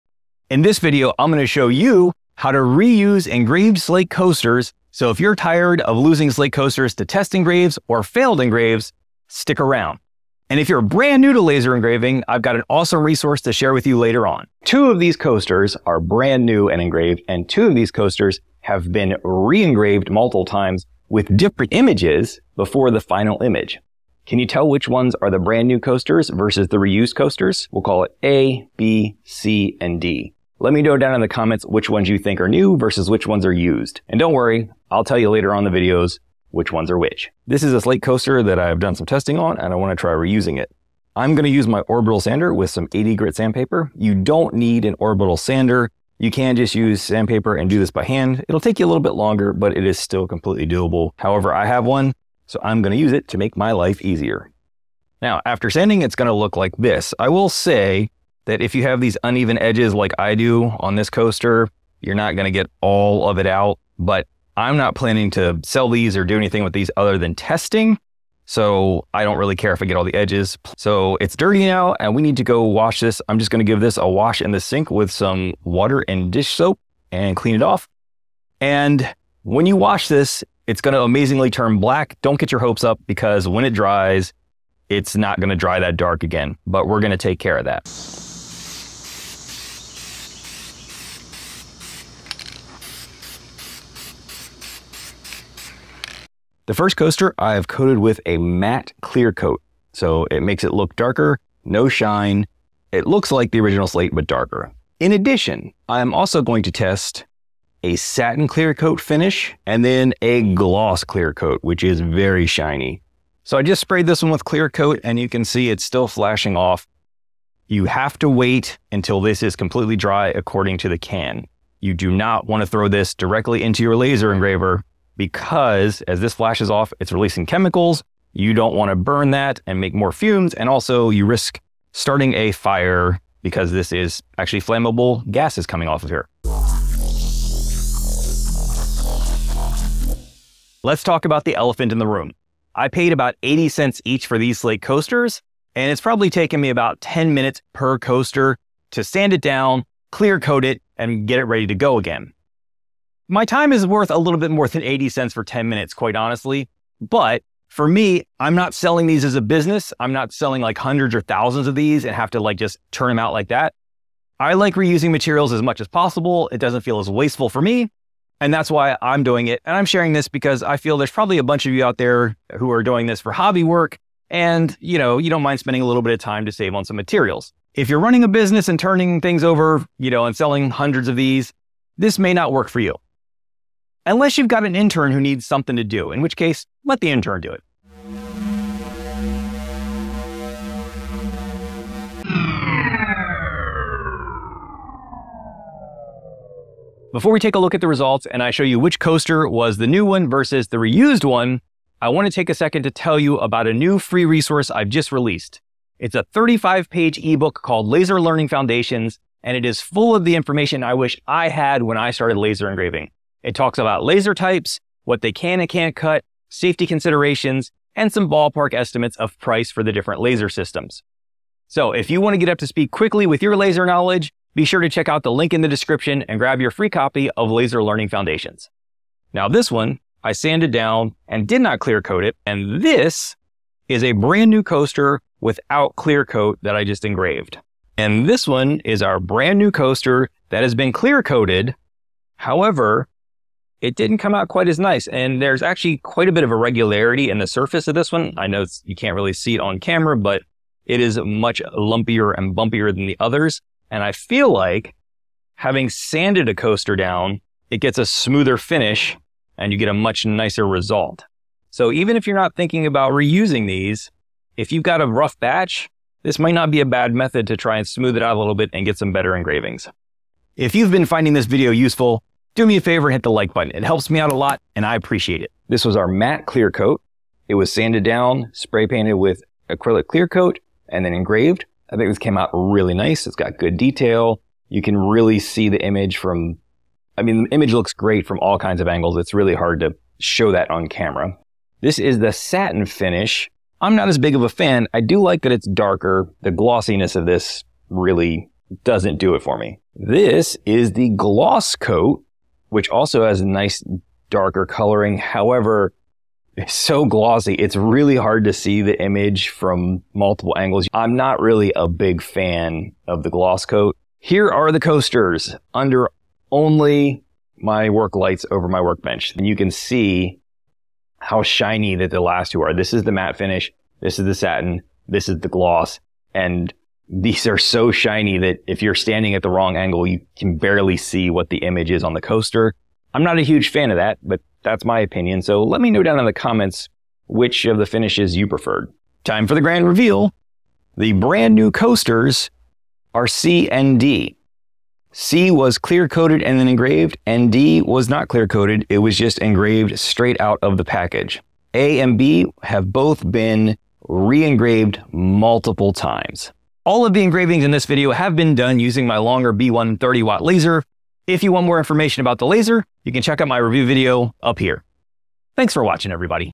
Stop Throwing Out Failed Slate Engravings!
Did you know that it's possible to re-engrave slate? In this video I show you how you can reuse slate coasters and test out a few different finishes.

If you buy using my links below I may get small share of the sale.

Longer B1 40 Watt Laser Engraver/Cutter:

Tools, Equipment, & Materials:
Twotrees Laser Enclosure 800x800x400
12-Pack Slate Coasters
Krylon Clear Coat

PPE:
OD 6+ Laser Safety Glasses